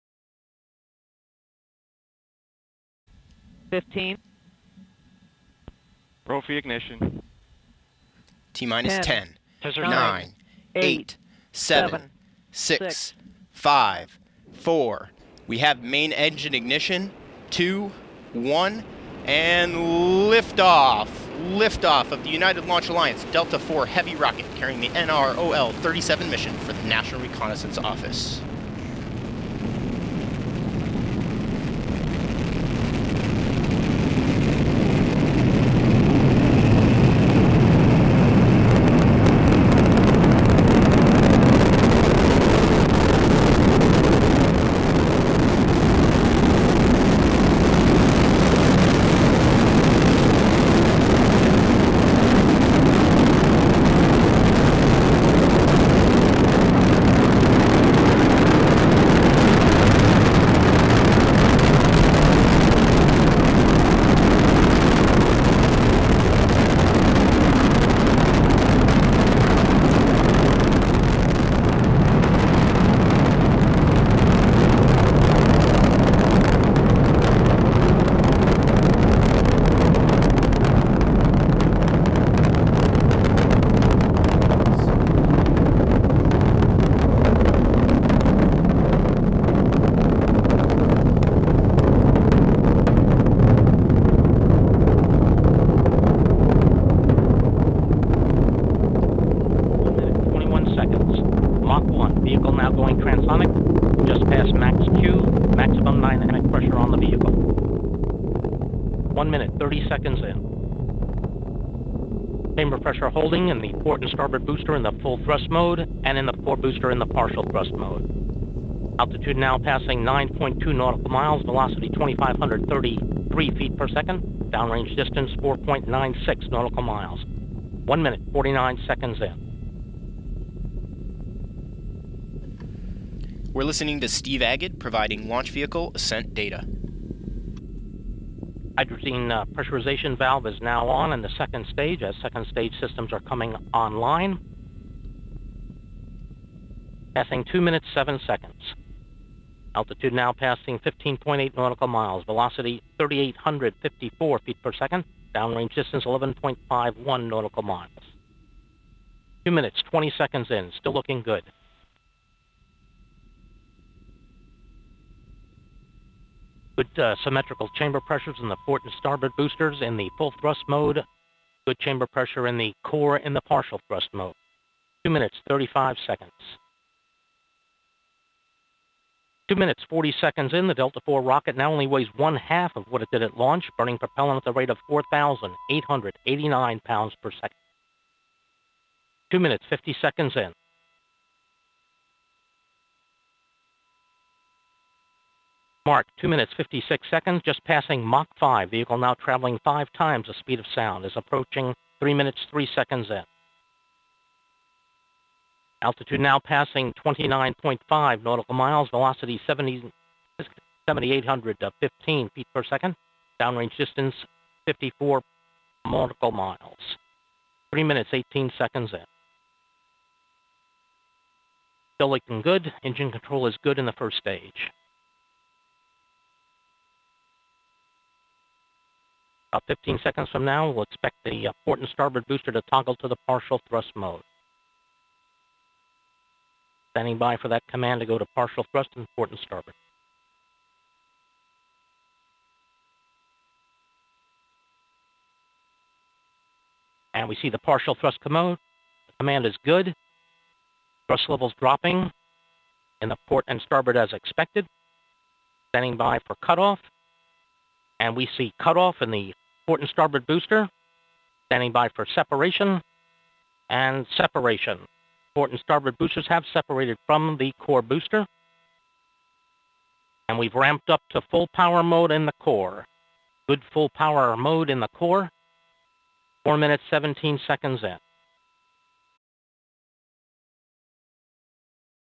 Delta IV Heavy launches NROL-37, 11 June 2016
A United Launch Alliance Delta IV Heavy rocket launched the NROL-37 mission for the National Reconnaissance Office (NRO), on 11 June 2016, at 17:51 UTC (1:51 p.m. EDT), from Space Launch Complex (SLC)-37 at Cape Canaveral Air Force Station, Florida. NROL-37 was the 32nd Delta IV mission, and the 10th NRO mission to launch on Delta IV.

Credit: United Launch Alliance